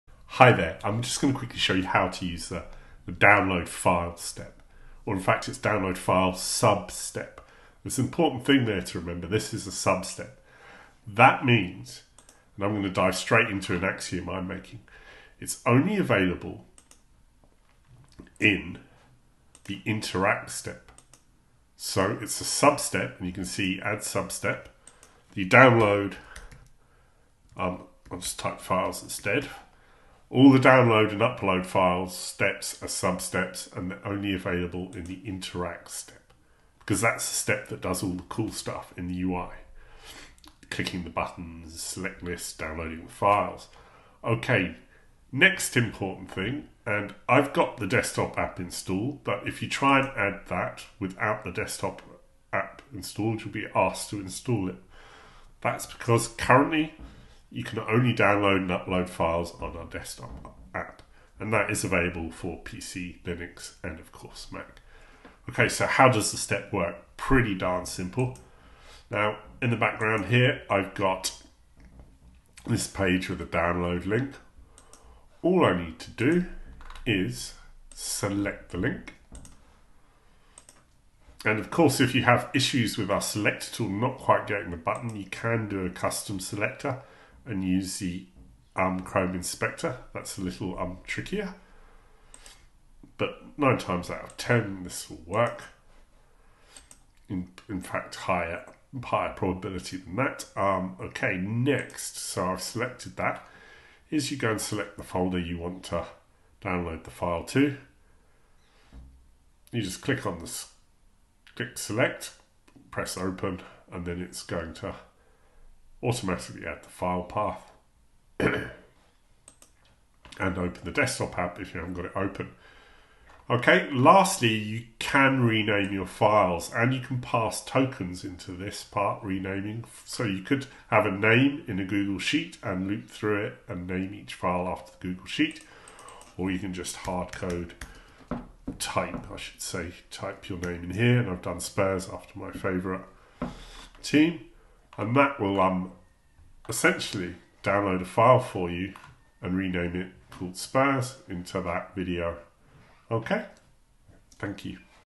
How to use the Download Files step
Learn how to use the Download File step using Axiom's no code bot builder.

0:00 - How to us the Download File step
0:14 - The Download File step is a sub-step
0:44 - The Download File step requires the desktop application
1:10 - How to use the Download File sub-step
2:07 - How to rename files